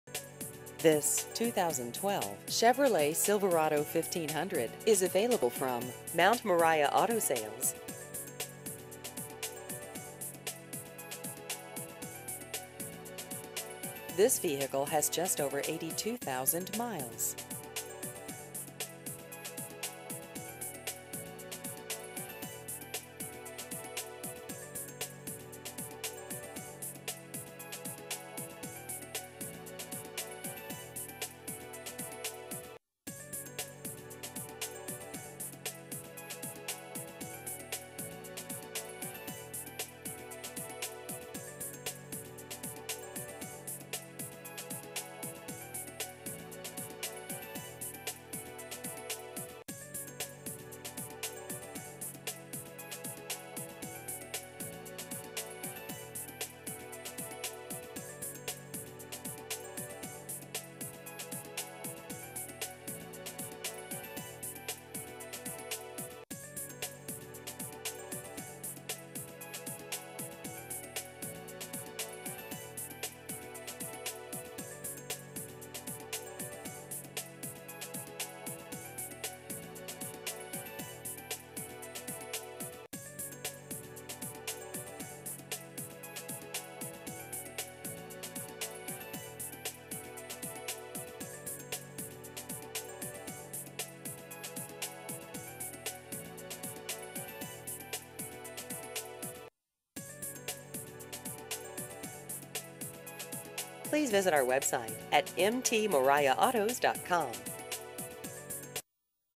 2012 Chevrolet Silverado 1500 Used Cars Memphis TN
This 2012 Chevrolet Silverado 1500 is available from Mt. Moriah Auto Sales.